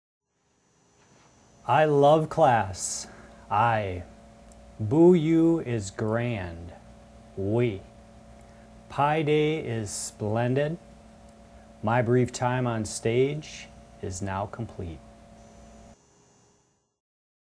UW Baraboo/Sauk County Pi Day Pi-Ku inspirations: Paul: Frisbee
Paul gets inspiration for a Pi-Ku (thanks, Gabe)